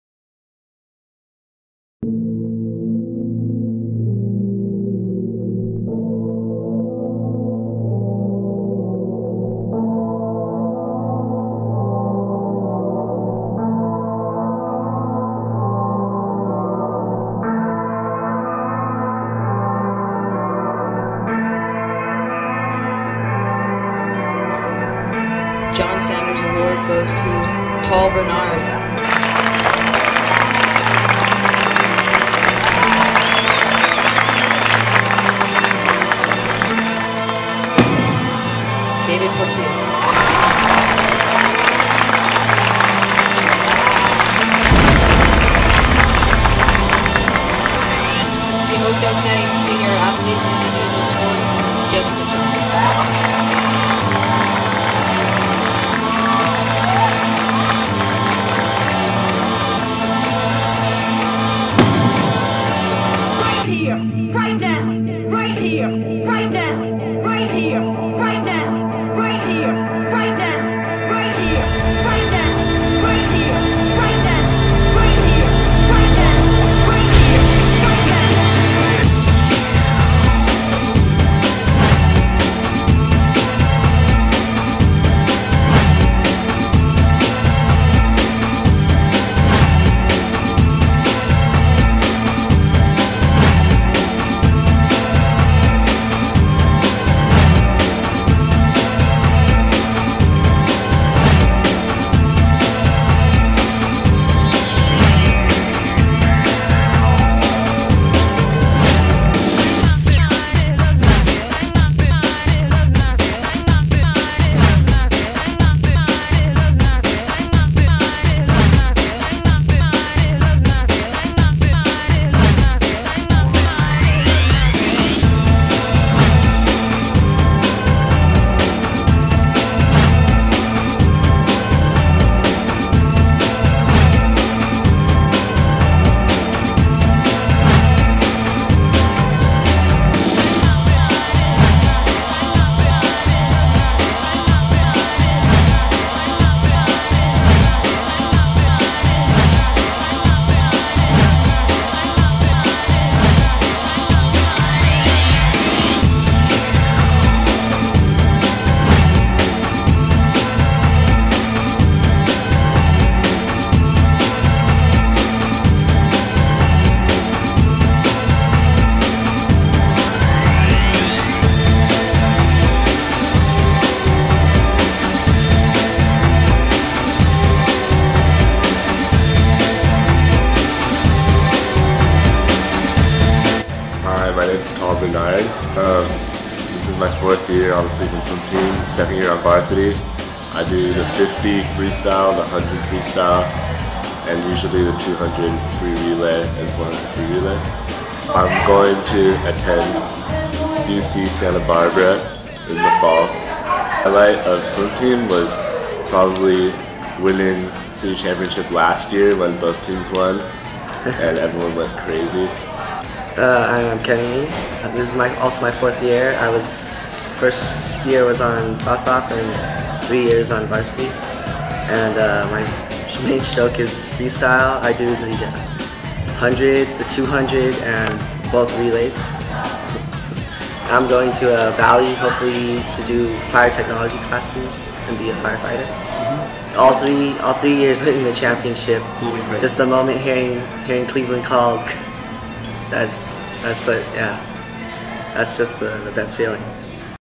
Cleveland High School Swim Team Seniors 2010 (Part I of II)
Reflecting back on 4 years on the greatest HS swim team program.
The friends and highlights and the camaraderie that will last long after you all graduate.